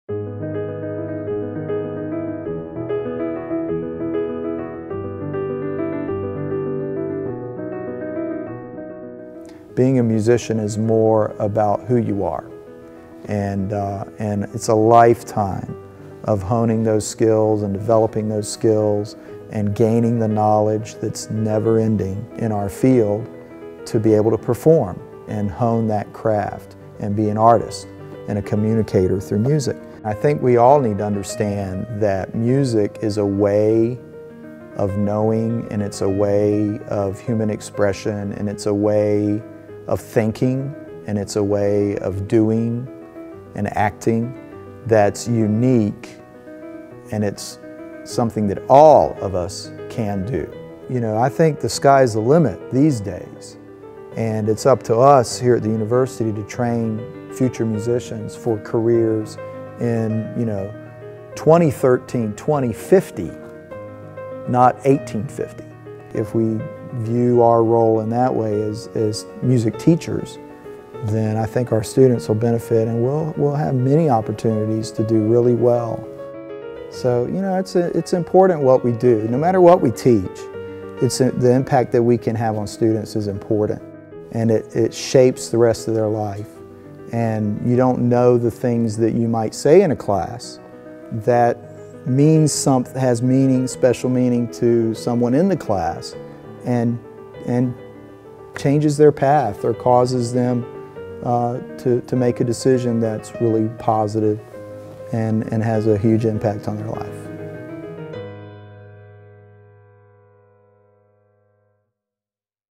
Meet the Faculty: Wayne Gallops
Wayne Gallops, an associate professor of music, believes that music is a way of human expression that all of us can do.